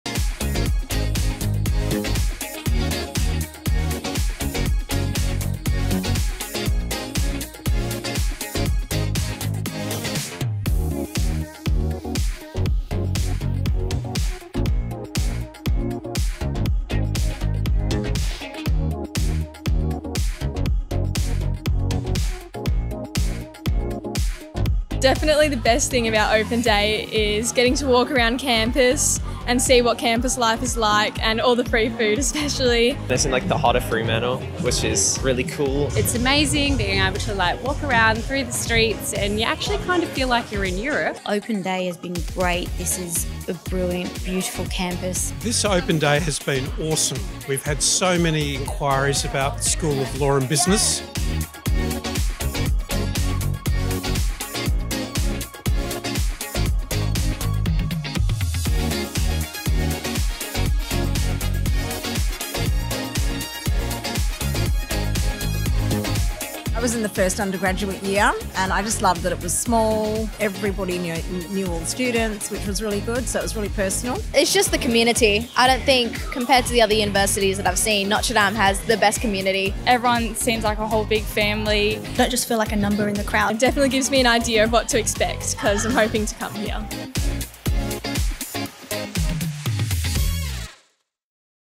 Notre Dame University – Fremantle Open Day 2024
Thank you for joining us on 4 May for a fantastic Open Day!

We trust it gave you a sense of the amazing community we have here at Notre Dame, our beautiful campus and a taste of the different areas of study on offer.

You will have met our dedicated academic staff and awesome student ambassadors, plus seen the array of clubs and student support services we're pleased to provide.

Choosing the right university for you is a big decision to make and we hope that Open Day helped to answer some of your questions.

We can’t wait to welcome you back as students!